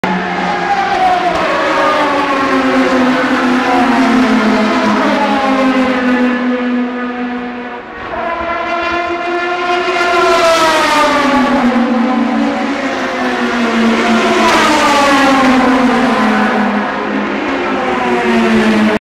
Nigel Mansell Ginetta Zytek Red 5 Le Mans Series Silverstone 2009 1
This is a video of Nigel Mansell driving the Ginetta Zytek Red 5 at the Le Mans Series Silverstone 2009 event.
This is video 1 of 2. I filmed this at the Start finish staight just after the start.
Please look at my other videos, including;
Bugatti Veyron,  Morgan, Ginetta,  Zytek,  Audi Quattro.